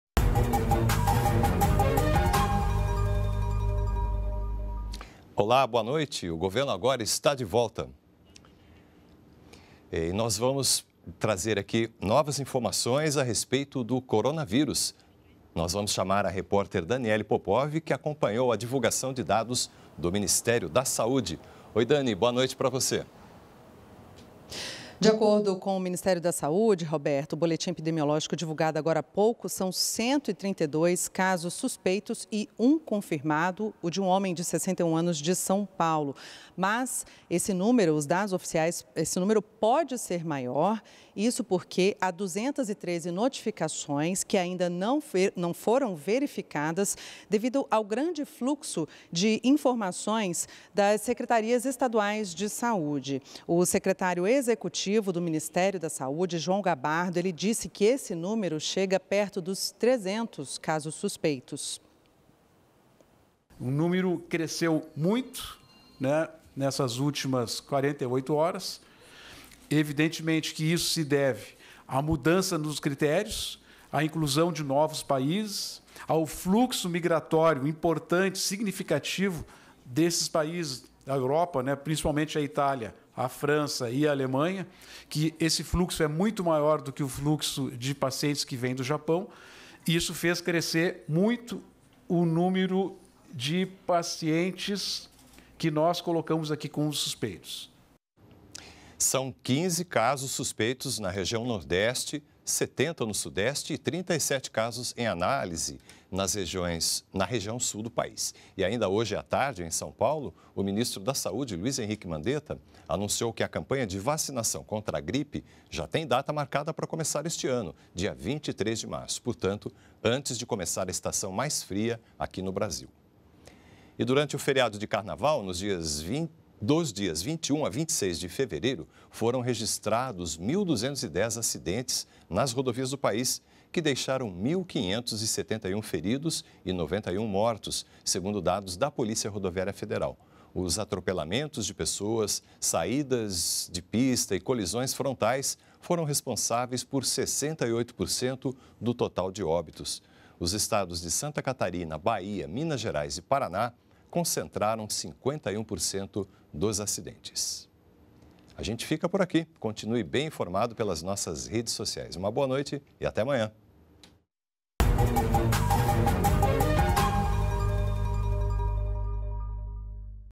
Ministério da Saúde investiga 132 casos suspeitos de coronavírus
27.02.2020 - O Ministério da Saúde atualizou para 132 o número de casos suspeitos do novo coronavírus no Brasil. A quantidade pode aumentar com a análise de outras 213 notificações. O feriado de Carnaval registrou 1.210 acidentes nas rodovias do país, com 1.571 feridos e 91 mortos, segundo dados da Polícia Rodoviária Federal.